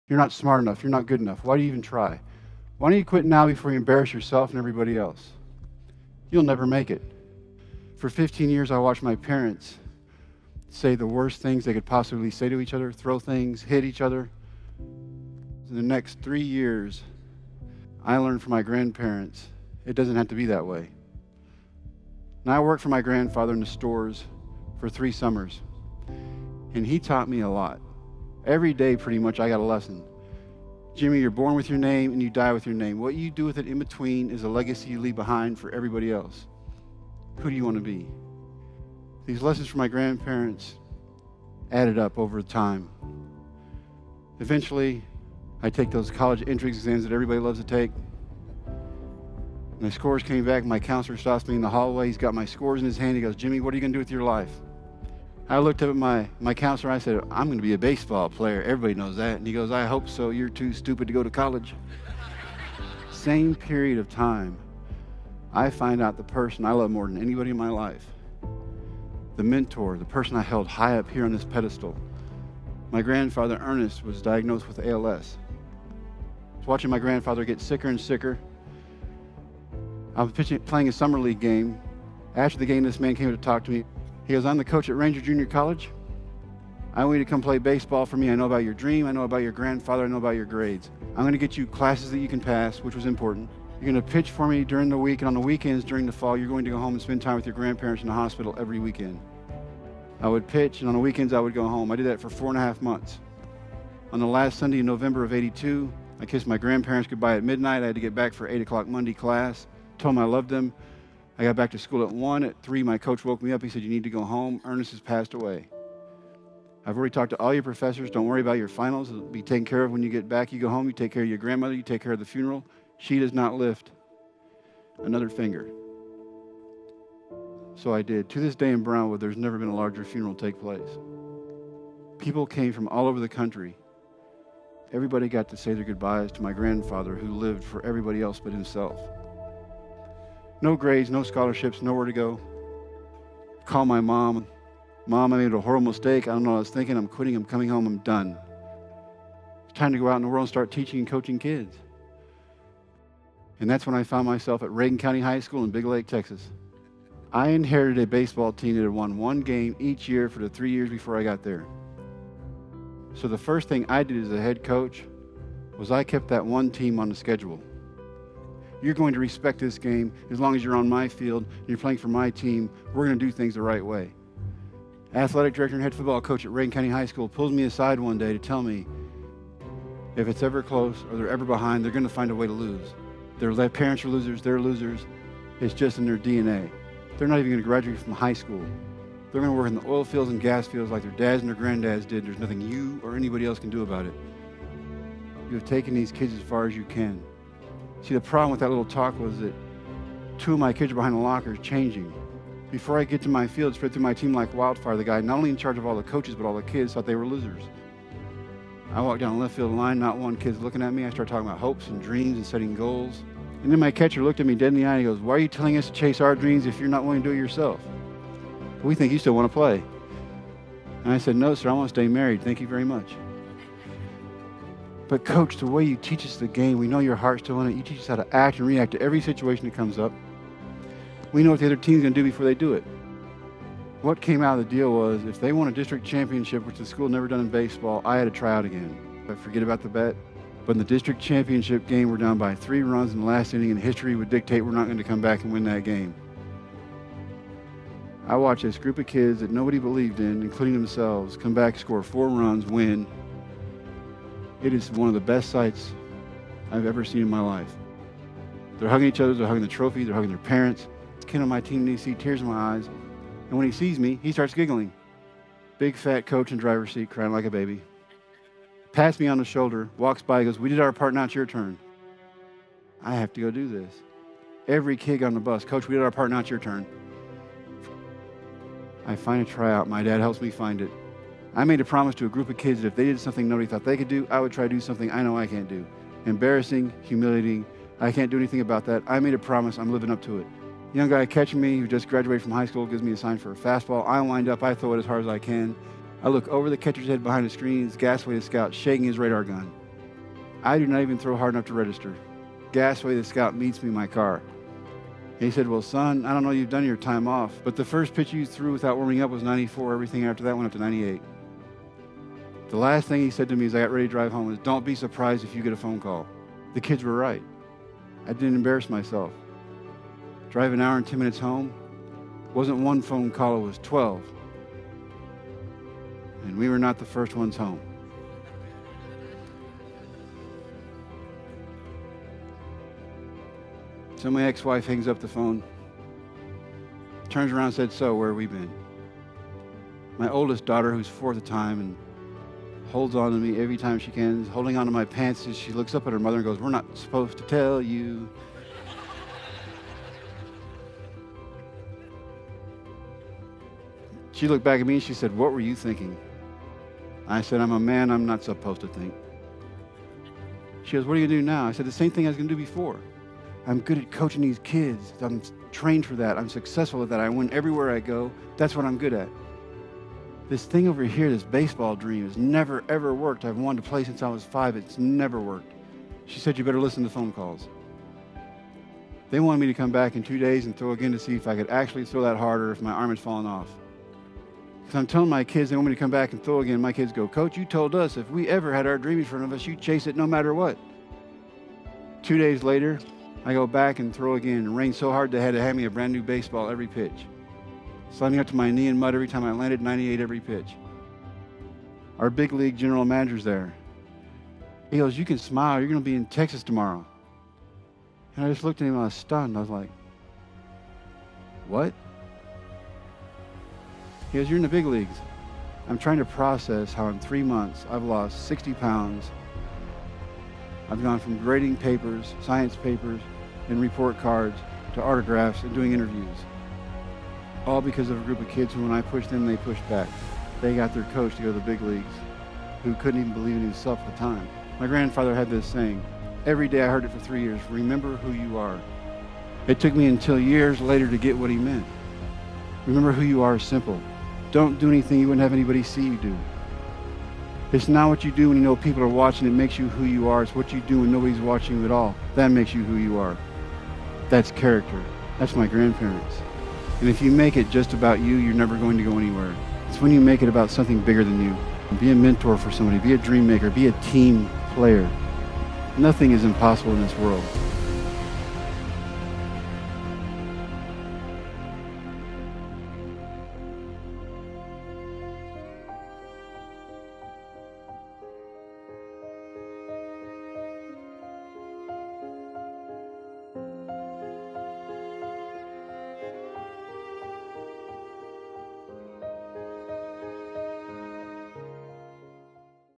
The Unbelievable True Story of Baseball's Oldest Rookie | Jim Morris Motivational Speech | Goalcast
Jim Morris' life was so remarkable, they made a Disney movie out of it. This is the unbelievable true story of how one man made the big leagues at the age of 35.

Full transcript for the Jim Morris speech in the description below.

Full Transcript - The Unbelievable True Story of Baseball's Oldest Rookie | Jim Morris | Goalcast

You’re not smart enough. You’re not good enough. Why do you even try? Why don’t you quit now before you embarrass yourself and everybody else? You’ll never make it.

For 15 years, I watched my parents say the worst things they could possibly say to each other, throw things, hit each other. The next three years, I learned from my grandparents it doesn’t have to be that way.

I worked for my grandfather in the stores for three summers, and he taught me a lot. Every day pretty much, I got a lesson. Jimmy, you’re born with your name and you die with your name. What you do with it in between is a legacy you leave behind for everybody else. Who do you want to be?

These lessons from my grandparents added up over time. Eventually, I took those college entry exams that everybody loves to take, and the scores came back and my counselor stops me in the hallway. He’s got my scores in his hand. He goes, “Jimmy, what are you going to do with your life?” I looked at my counselor and I said, “I’m gonna be a baseball player. Everybody knows that.” He goes, “I hope so. You’re too stupid to go to college.”

Same period of time, I find out the person I love more than anybody in my life, the mentor, the person I held high up here on this pedestal, my grandfather, Ernest, was diagnosed with ALS. No grades, no scholarships, watching my grandfather get sicker and sicker.

I was playing a summer league game. After the game, this man came to talk to me. He goes, “I’m the coach at Ranger Junior College. I want you to come play baseball for me. I know about your dream. I know about your grandfather. I know about your grades. I’m gonna get you classes that you can pass,” which was important, “you’re gonna pitch for me during the week, and on the weekends during the Fall, you’re going to go home and spend time with your grandparents in the hospital every weekend.” I would pitch, and on the weekends, I would go home. I did that for 4-1/2 months. On the last Sunday in November of ’82, I kissed my grandparents good-bye at midnight. I had to get back for eight o’clock Monday class, told them I loved them. I got back to school at 1:00. At 3:00, my coach woke me up. He said, “You need to go home. Ernest has passed away. I’ve already talked to all of your professors. Don’t worry about it. Your finals will be taken care of when you get back. You go home. You take care of your grandmother. You take care of the funeral. She does not lift another finger.” So I did.

To this day, in Brownwood, there’s never been a larger funeral take place. People came from all over the country. Everybody got to say their good-byes to my grandfather, who lived for everybody else but himself.
No grades, no scholarships, nowhere to go. Call my mom. “Mom, I need a horrible mistake. I don’t know what I was thinking. I’m quitting. I’m coming home. I’m done. It’s time to go out in the world and start teaching and coaching kids.”

That’s when I found myself at Reagan County High School in Big Lake, Texas. I inherited a baseball team that had won one game each year for the three years before I got there. The first thing I did as a head coach was I kept that one team on the schedule. “You’re going to respect this game. As long as you’re on my field, you’re playing for my team. We’re going to do things the right way.” Athletic director and head football coach at Reagan County High School pulled me aside one day to tell me, “If it’s ever close or they’re ever behind, they’re gonna find a way to lose. Their parents are losers. They’re losers. It’s just in their DNA. They’re not even gonna graduate from high school. They’re gonna work in the oil fields and gas fields like their dads and their granddads did, and there’s nothing you or anybody else can do about it. You have taken these kids as far as you can.”